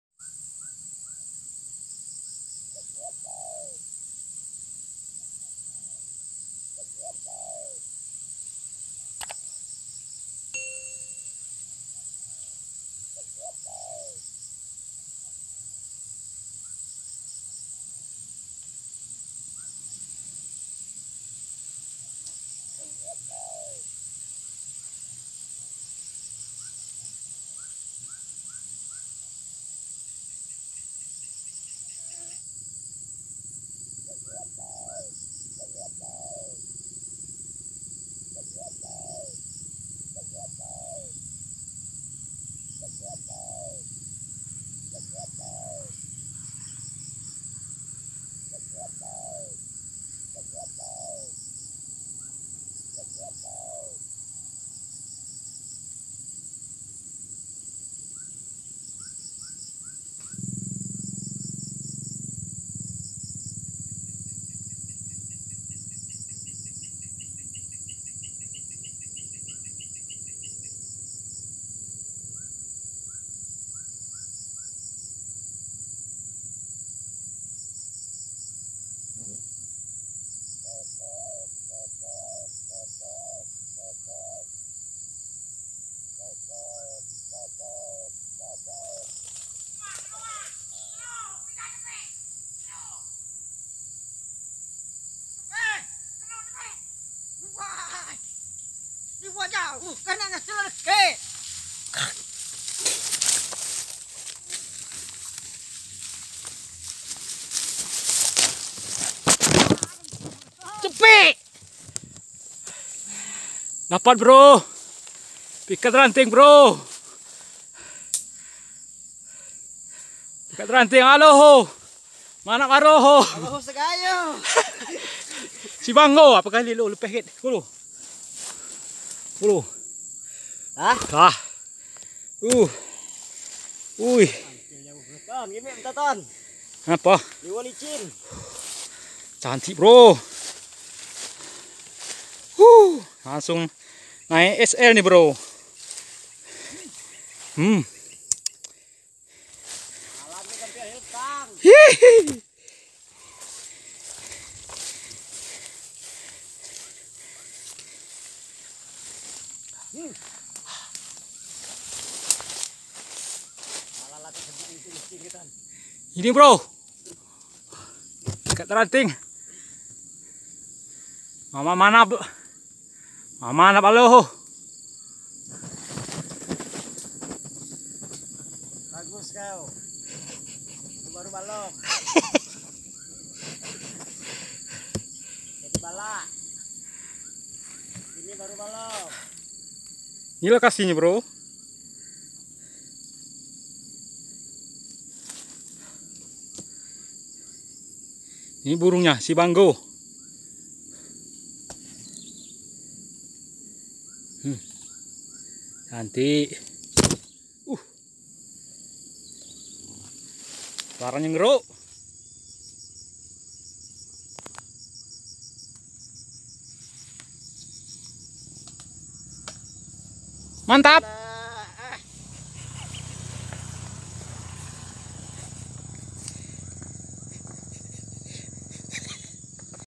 Pikat ranting MAMAK MANAF ALOHO. ~TERKUKUR TABIR
Salam TERKUKUR TABIR
dalam perjalanan pulang dari lokasi mikat kita ketemu BL di pinggir jalan, BL nya banggo sura besar ngeruk kita pasang pikat ranting mamak manaf karena di lokasi sebelumnya mamak manaf zonk.

record: Hp samsung galaxi a6
editing video: kinemaster
editing thumnail: kinemaster